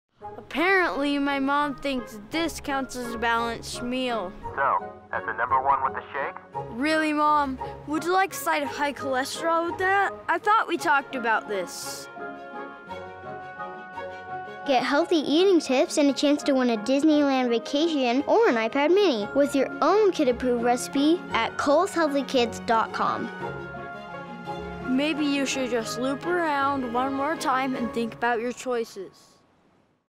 Drive Thru - Kohl's Healthy Kids 30 sec Commercial
At kohlshealthykids.com your kids might learn to like nutritious foods better than you do. And they just might teach you a thing or two about healthy eating.

Directed by Tyler Sohm

Crew:
Director of Photography/Editor/Colorist: Bryan Thacker
First Assistant Camera: Luke Jensen
Gaffer: Tyler Meiners
Production Sound Mixer: Conrad Laga
Audio Engineer: Shaun Boulter